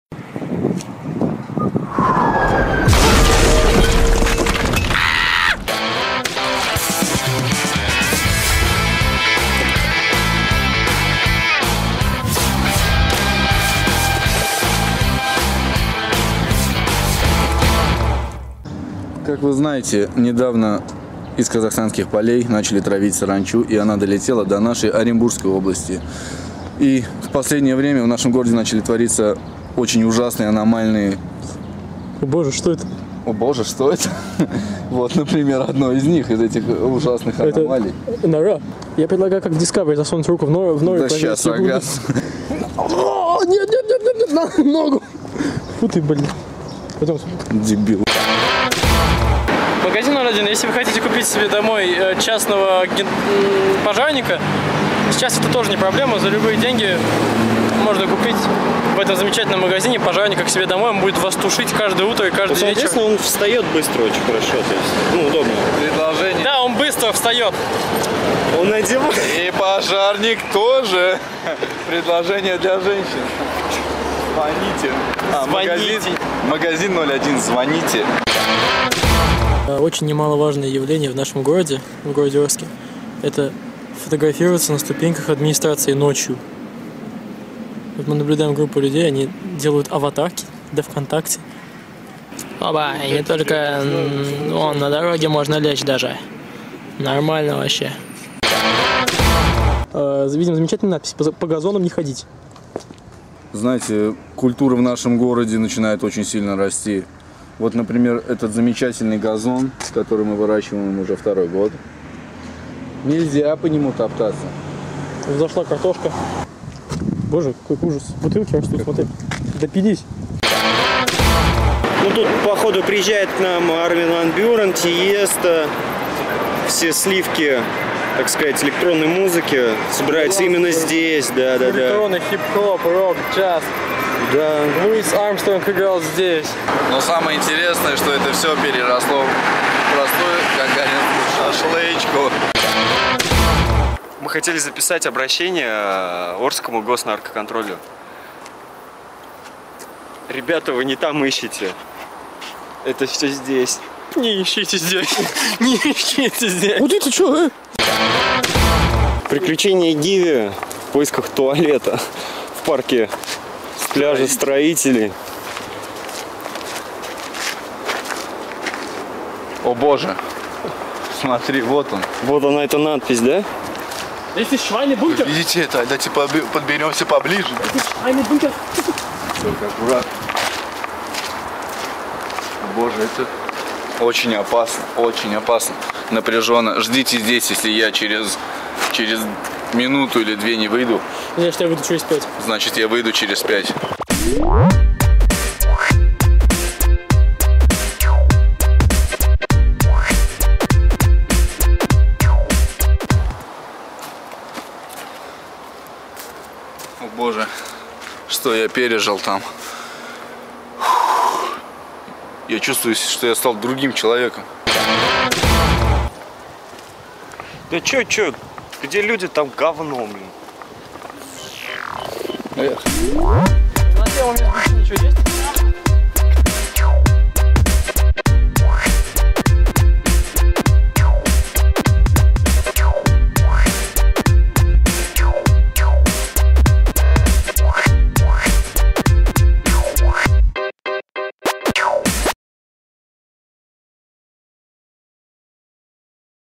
Орские аномалии Изнутри 5
Вы знаете, где в Орске скрыты аномальные места в виде «черных дыр», загадочных письмен на стенах и прочих паранормальных явлений? Эти места чрезвычайно опасны, они способны неожиданно засосать верхнюю или нижнюю конечность мирного прохожего или кардинально изменить ваше сознание за пару секунд!

Мы предупреждаем вас обо всех аномальных опасностях. Смотрите новую видеоэкскурсию проекта «Изнутри»

Свежие выпуски проекта «Изнутри» смотрите каждую неделю! Только на сайте Урал56.Ру.